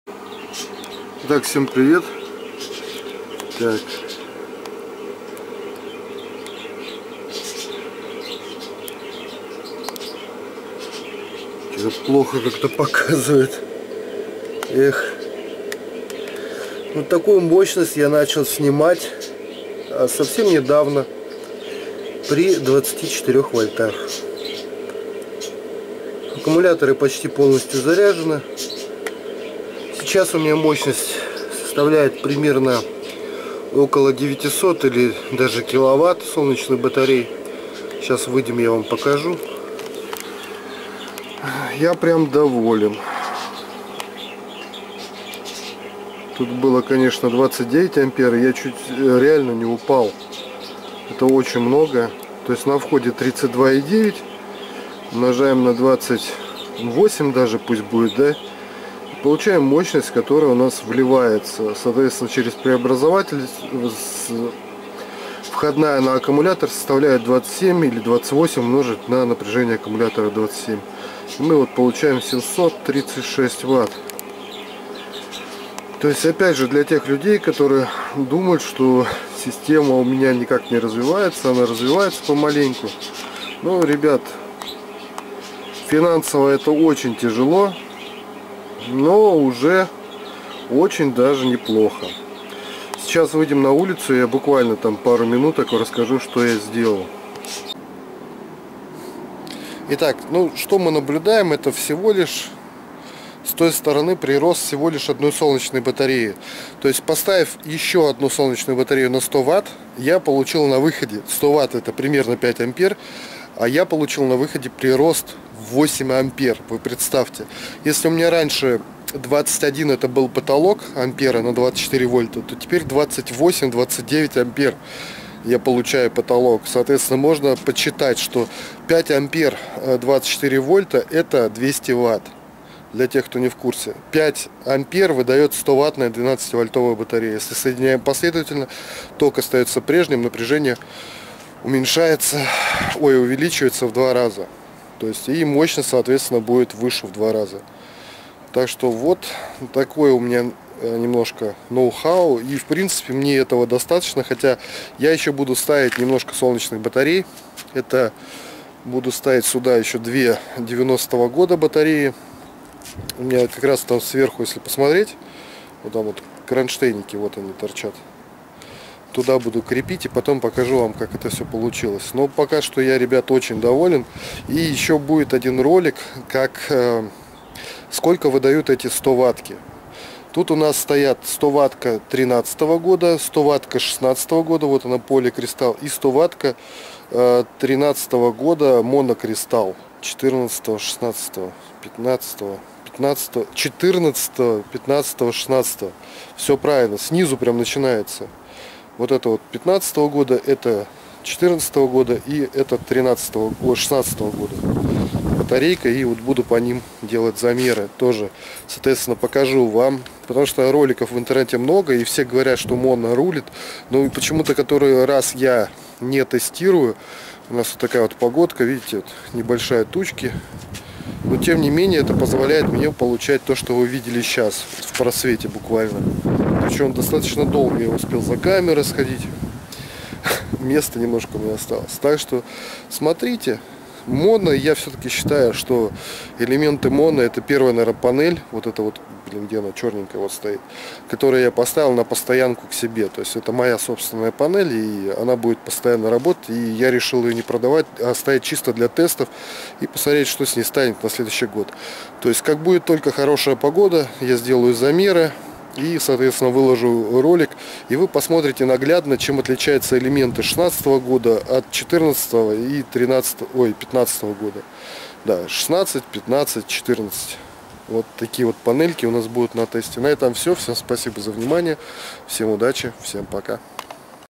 Добавил одну солнечную панель получил выше мощность чем рассчитывал, новые данные
Итак, решил закрыть оставшийся просвет на направляющих, еще одной солнечной панелью на 100 Ватт. И в итоге, у меня в этом году установлено 4 новых солнечных панели следующих годов выпуска: 2013г — 50ватт поли Германия, 2014г -100ватт моно Китай, 2015г -100ватт поли Китай,2016г -100ватт поли Китай.  В ближайшие дни, когда установится ясная погода, произведу замеры солнечных панелей и запишу показания на каждой из трех стоваттных панелей для сбора статистики.